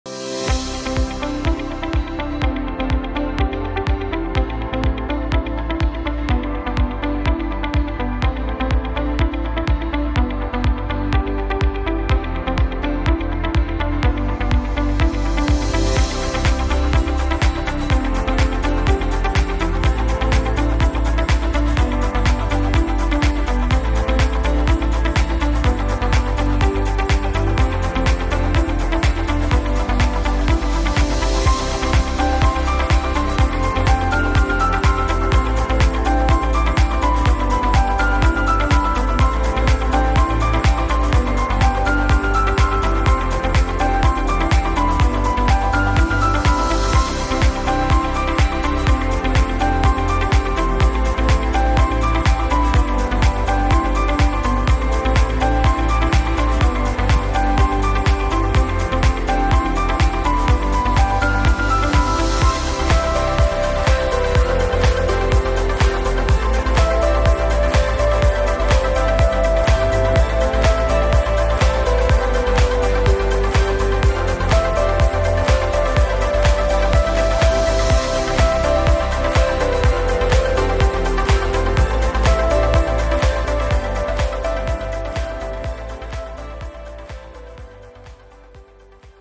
FPV Real Estate Demo Test
Testing the capabilities of an FPV drone to capture the outdoors of a $7.7M property located in Evergreen, CO.